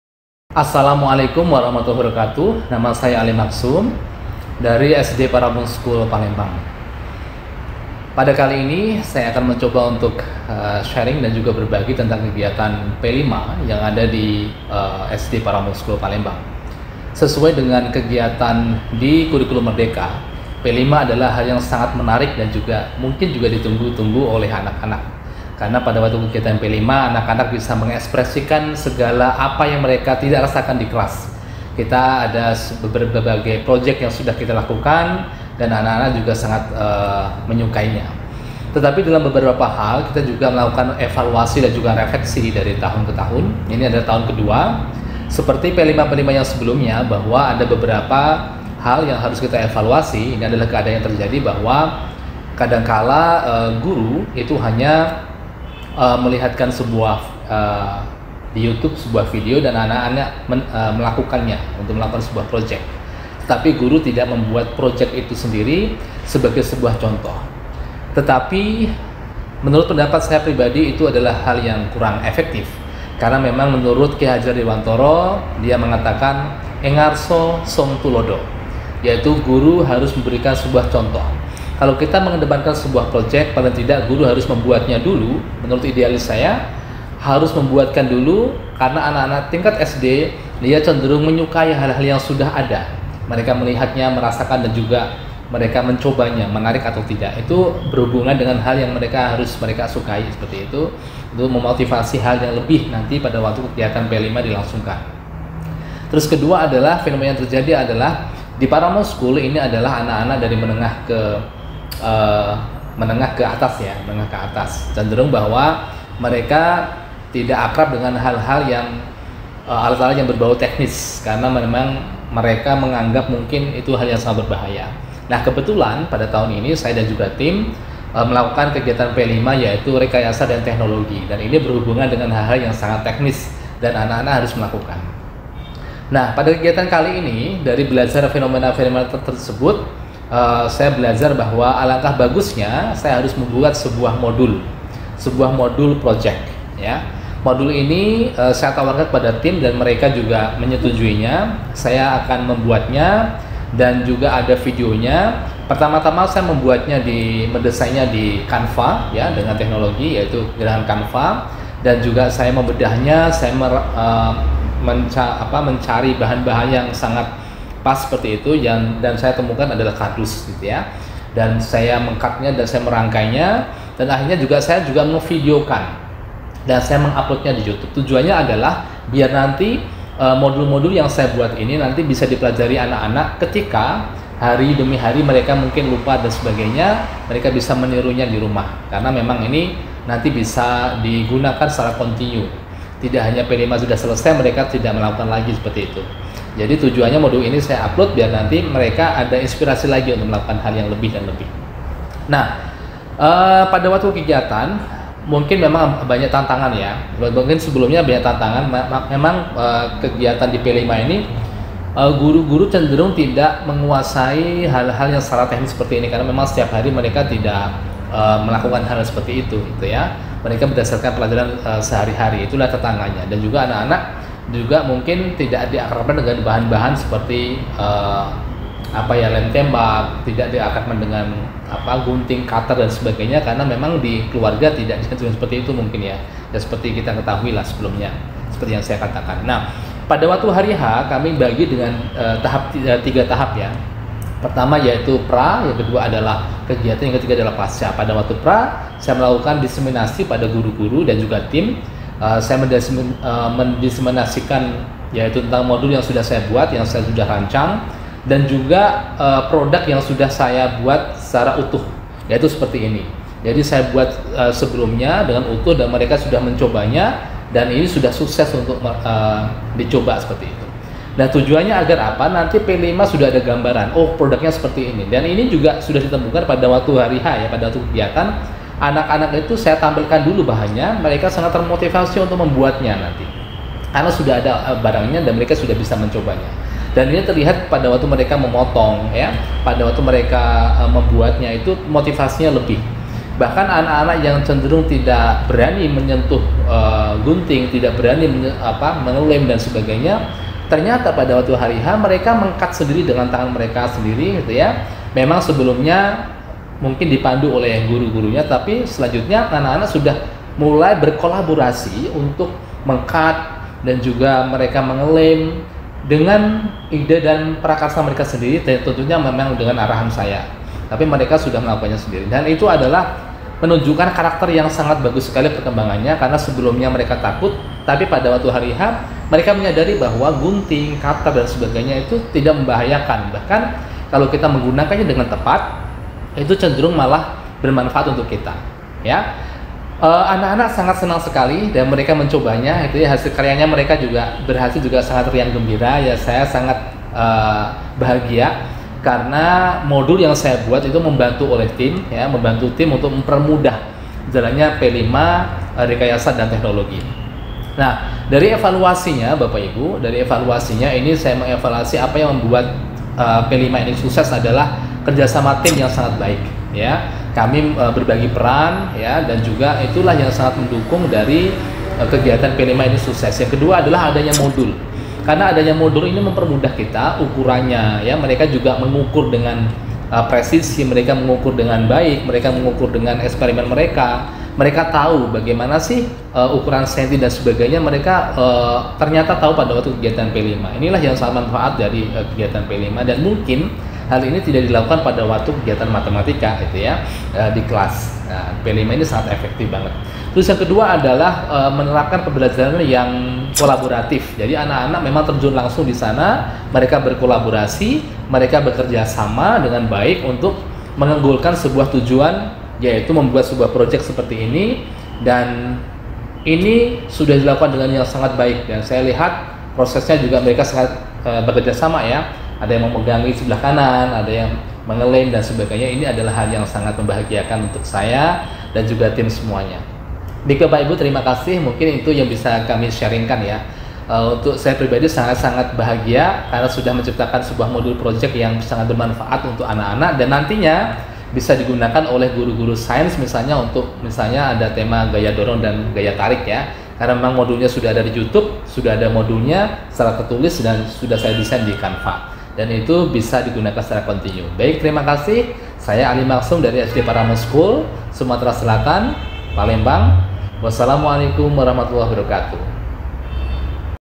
JAMBORE GTK HEBAT 2024: PROJEK P5 REKAYASA TEKNOLOGI
Video ini dibuta unttuk ikut berpartisipasi Jambore GTK Hebat 2024. Saya mengangkat kegiatan P5 SD Paramount School dg tema rekayasa dan teknologi.